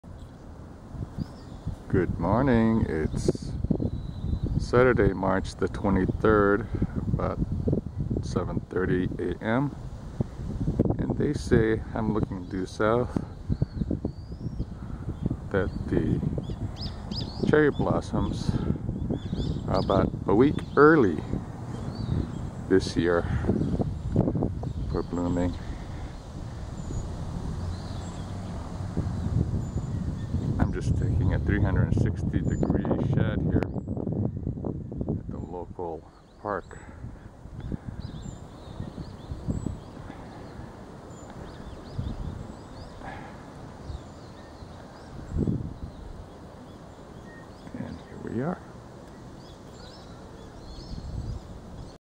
2013 Tokyo Cherry Blossoms - Fuchu
The cherry blossom bloom is about a week early this year.  Today the bloom is around 80% by next week the blossoms will start falling.  The mornings are tranquil.  The evenings are festive with people gathering for blossom viewing.